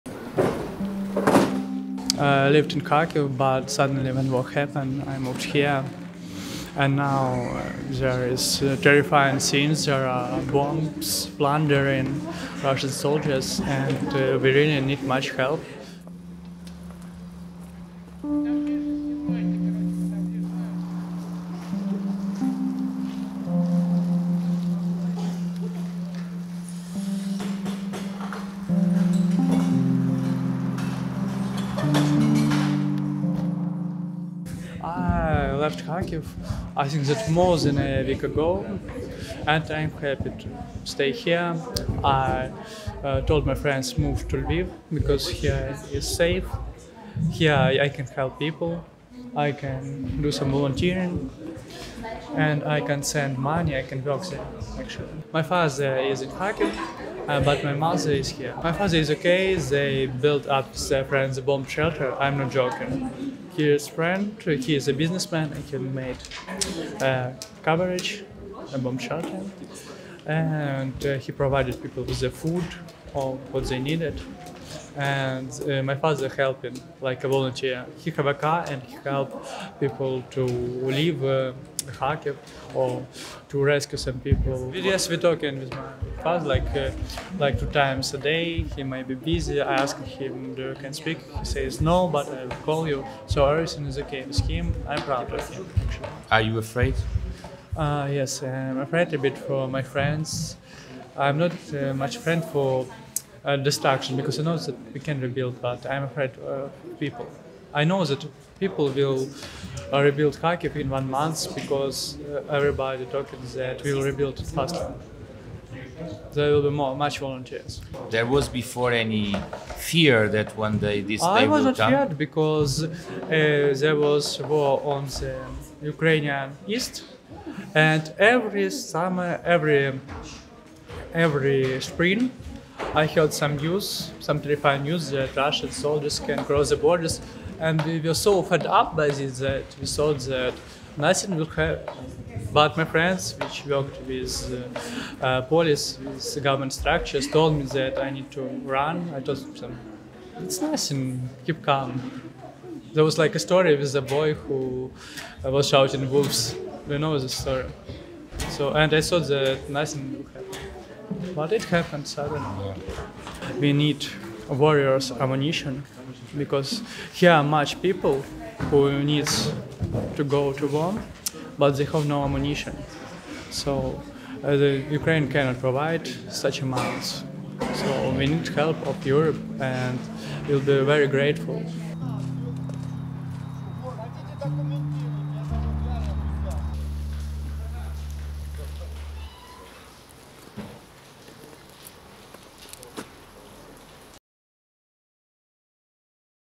Mykyta fugiu de Kharkiv quando os russos chegaram. “Tenho medo pelas pessoas”
Reportagem Renascença na Ucrânia
Tem 19 anos e veio de Kharkiv para Lviv com a mãe, quando os russos invadiram aquela cidade na fronteira leste da Ucrânia. Sobre o pai, que não fez a viagem para Lviv, surpreende-nos com o que faz: ajuda a construir abrigos anti bombardeamento. Para este jovem apesar de tudo há uma certeza. Pedra a pedra, a cidade de Kharkiv acabará por ser reconstruída. O problema são as pessoas que desaparecem.